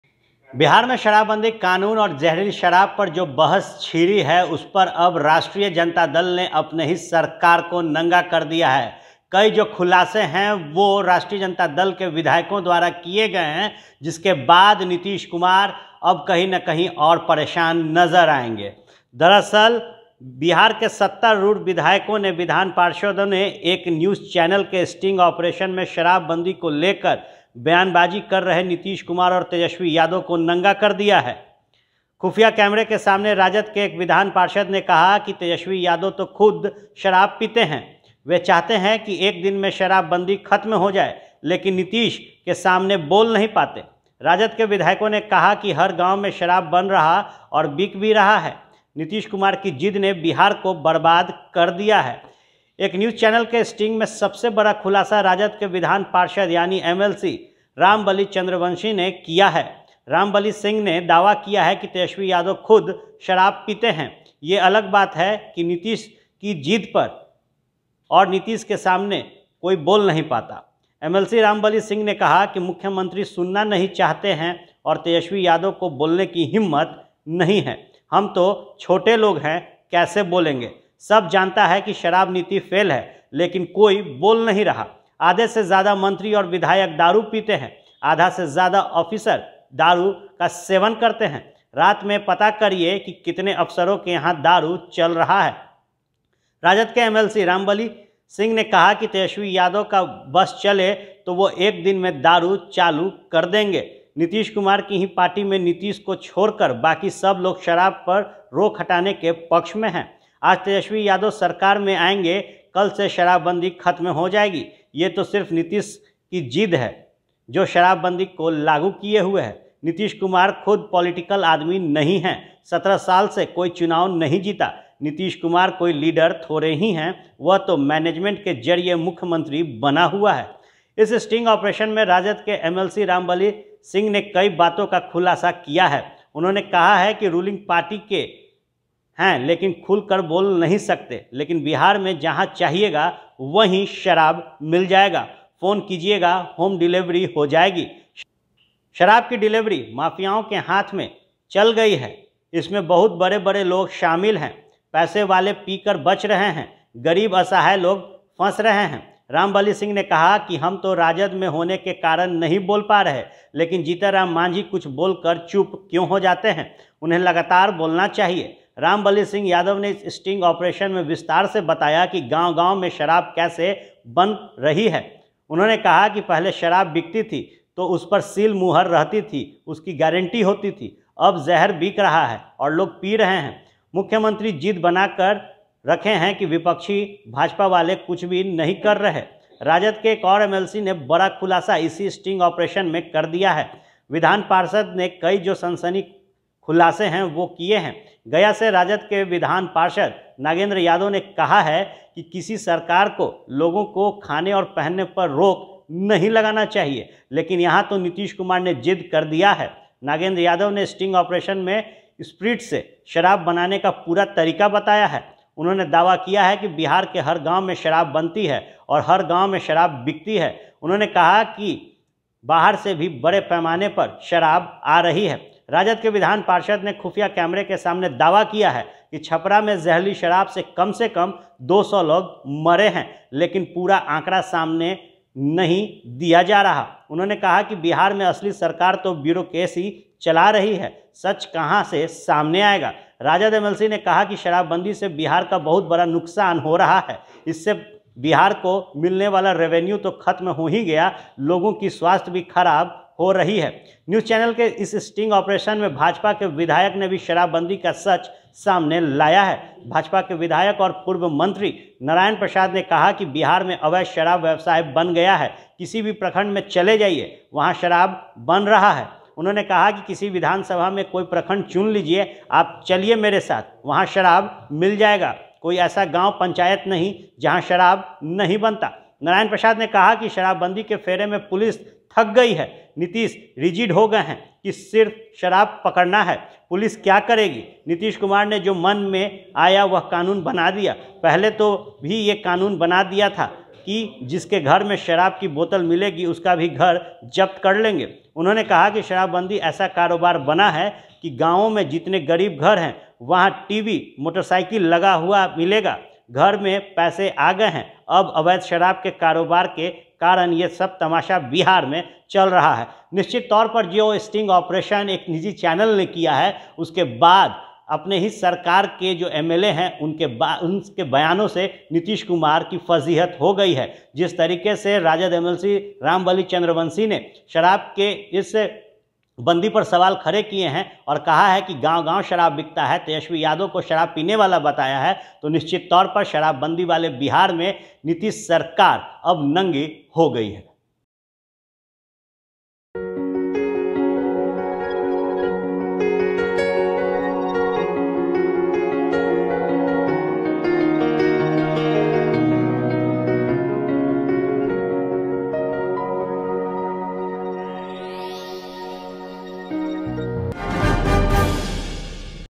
Tejaswi खुद पीते हैं.... Nitish से बोल नहीं पाते,देखिए स्टिंग ऑपरेशन
Other Trending Videos:

1. श राब पर दो महीने पहले ही पटना हाईकोर्ट ने नीतीश को चेताया था, फिर भी सोये रहे CM